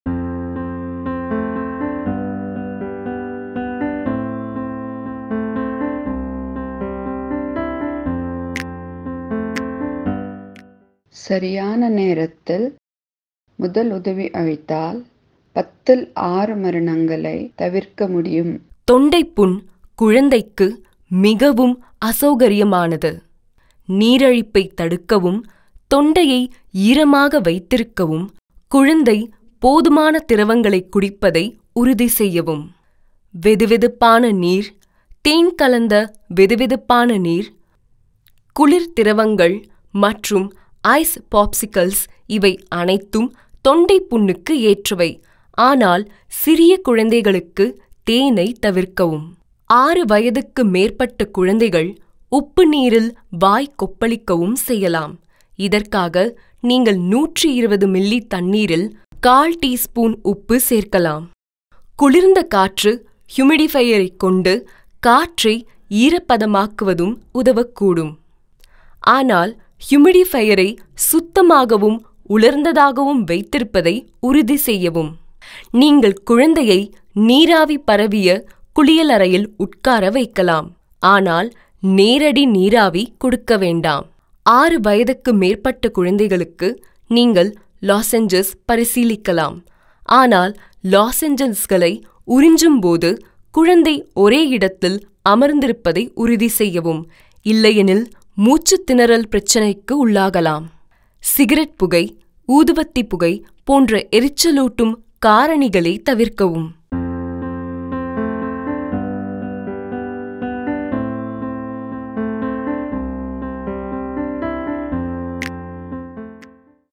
தொண்டை வலிக்கான முதலுதவி | KIMS Cuddles
முதலுதவி என்பது உங்கள் நண்பர்களையும் குடும்பத்தினரையும் பாதுகாப்பாக வைத்திருக்க ஒரு சிறந்த வழியாகும்.முதலுதவி கற்றுக்கொள்வது ஆபத்து அறிகுறிகளை அடையாளம் காணவும், அவசரநிலைகளைக் கையாள்வதில் உங்களைத் திறமையாகவும் மாற்றும்.மாற்றுகிறது. அதோடு உலகில் முதலுதவி மீதான மக்களின் கண்ணோட்டத்திலும் இது பெரிய மாற்றத்தைக் கொண்டுவரும்.10ல் 6 இறப்புகளை சரியான நேரத்தில் முதலுதவி செய்வதன் மூலம் தடுக்கலாம்! தொண்டை வலிக்கான முதலுதவி பற்றி அறிய இந்த வீடியோவைப் பாருங்கள்